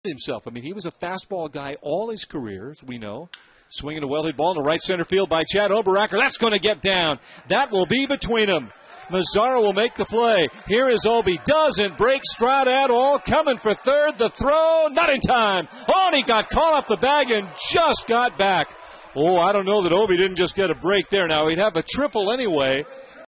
RockHounds' Oberacker ropes a triple
6/14/15: Midland's Chad Oberacker drives a triple to right in the RockHounds' 8-0 win over the Frisco RoughRiders